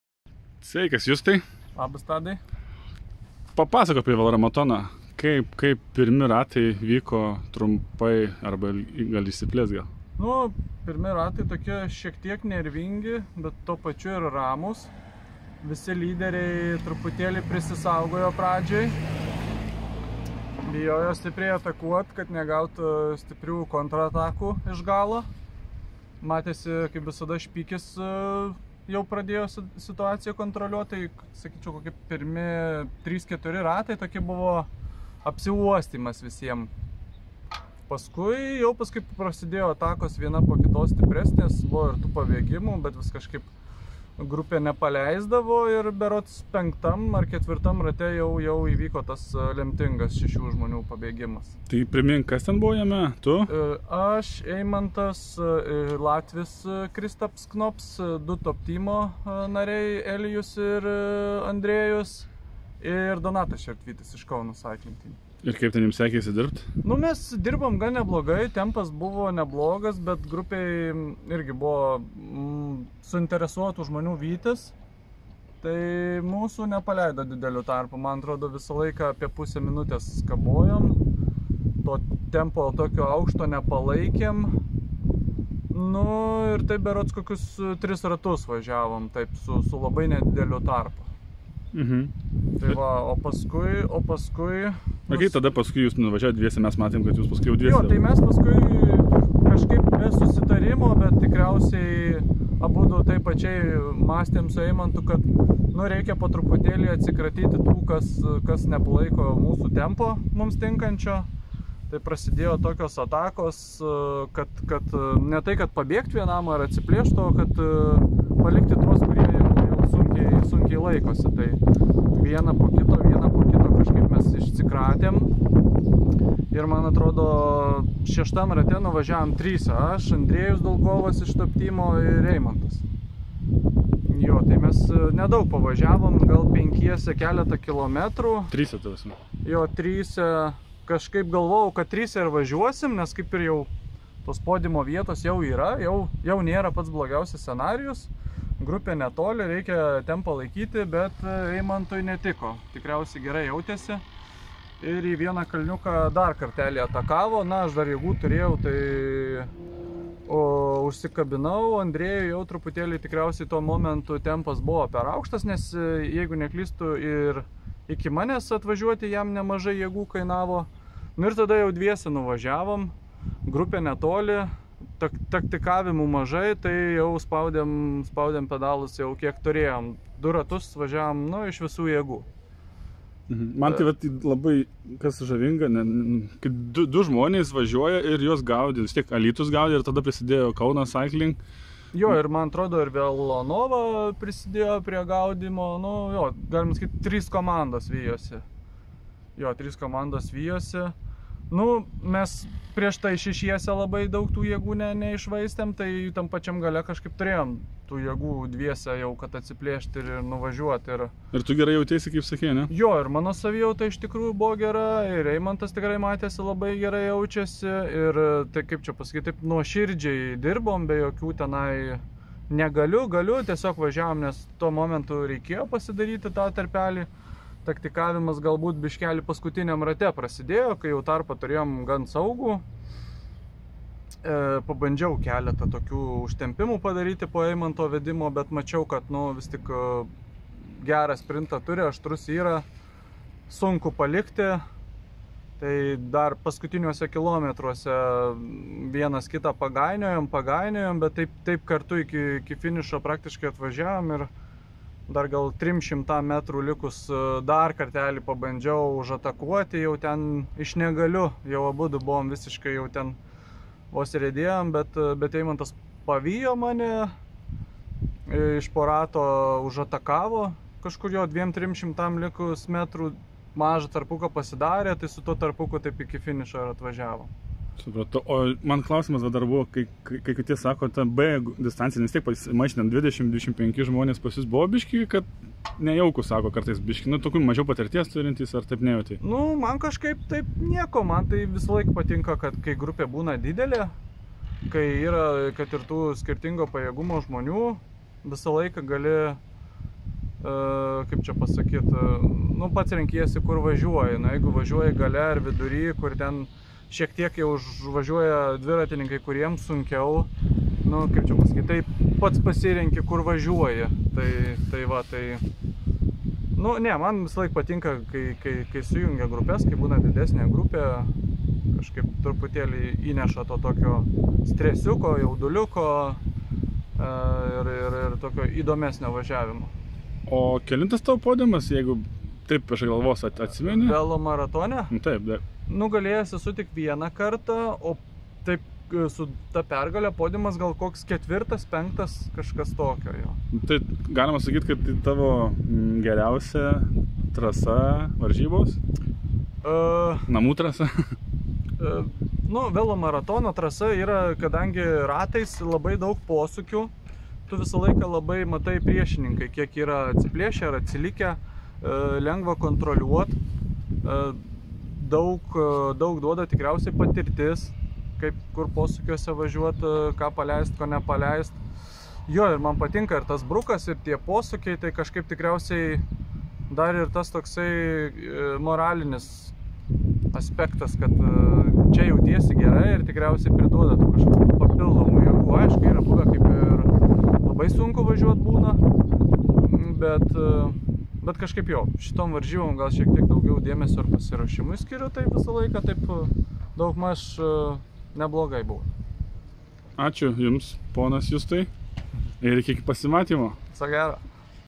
Justas Klimavičius apie IKI Velomaratoną - 2021
Justas pasakoja kaip vyko varžybos ikivelomaratonas 2021!

Reikia man jūsų paramos, kad darytume interviu ir kitą kontentą kokybiškesnį